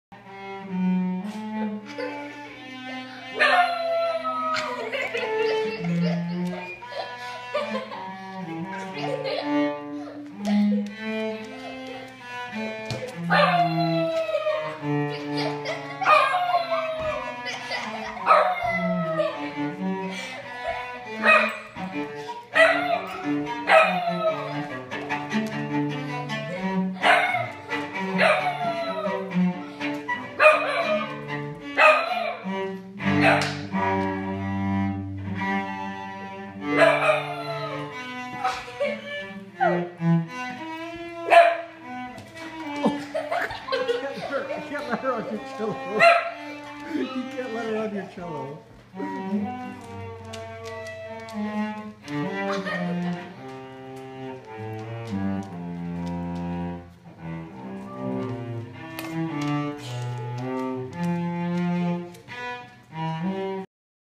Mini blue tic coonhound / corgi cross singing
Rosie sings along to cello tune